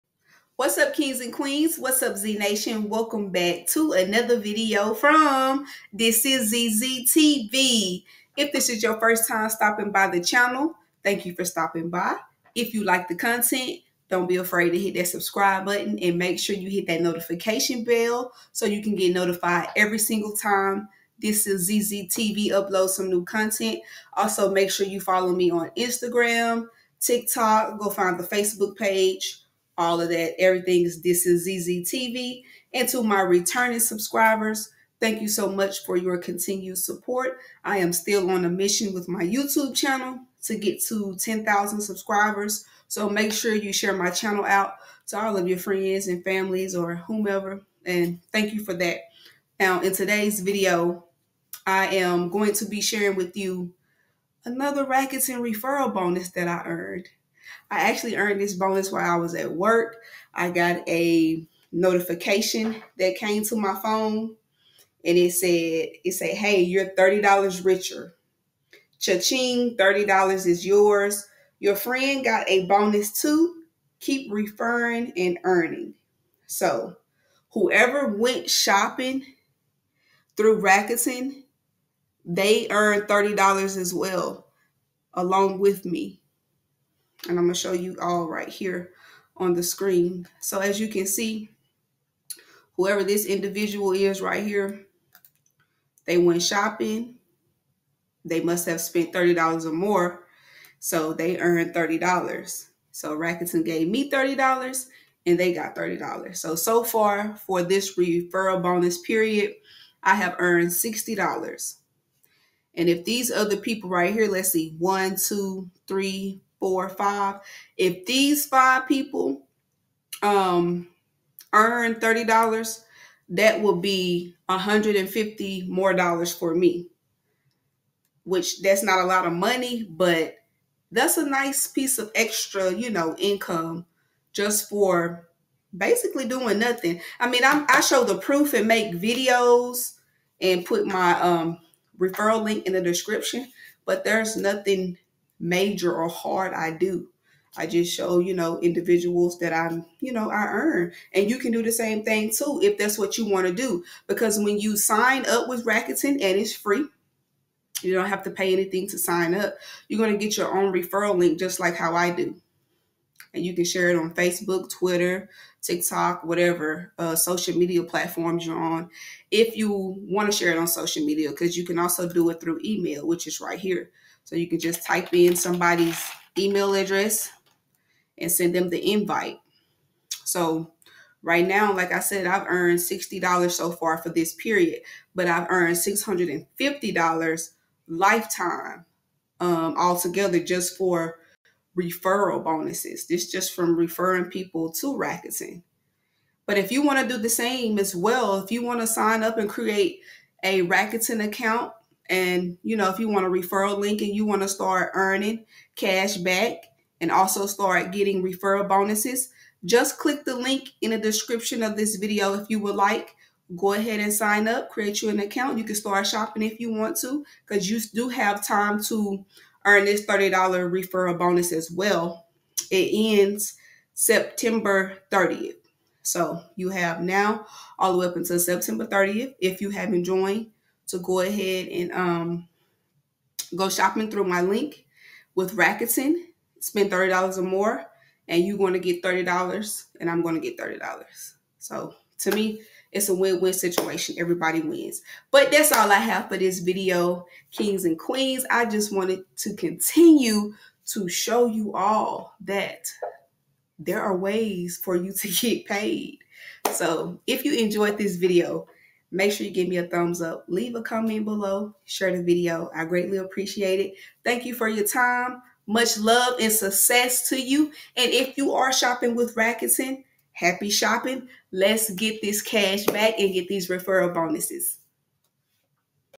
I Tried the Rakuten Referral Program (and it worked!)
In this video, we're diving into the Rakuten Referral Program. Discover how you can earn rewards by sharing the benefits of Rakuten with your friends and family. It's a win-win – they get cash back, and you get rewarded too!

Don't miss out on this fantastic opportunity to save while you earn with Rakuten.

👍 Drop a "LIKE" if you enjoyed the content